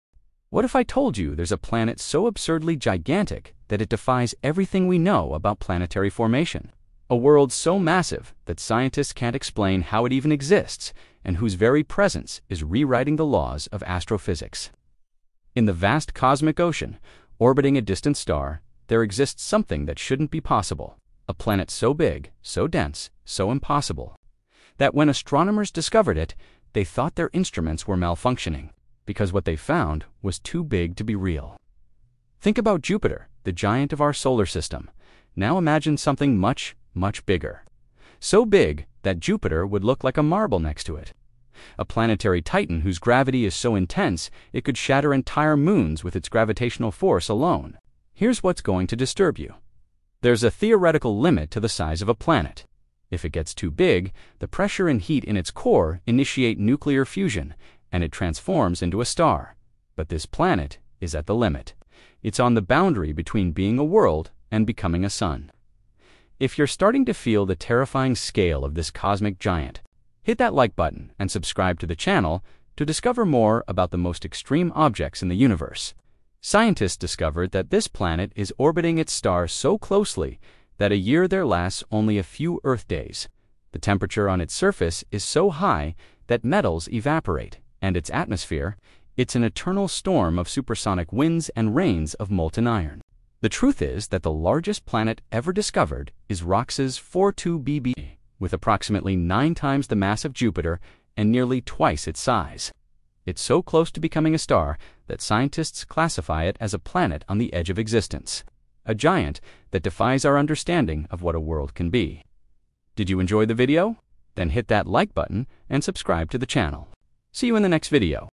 The Largest Planet in the Universe!
There's a planet so absurdly gigantic that scientists can't explain how it even exists.

A world so massive it's rewriting the laws of astrophysics.

This cosmic giant is on the edge between being a planet and becoming a star, and what happens on its surface is pure terror.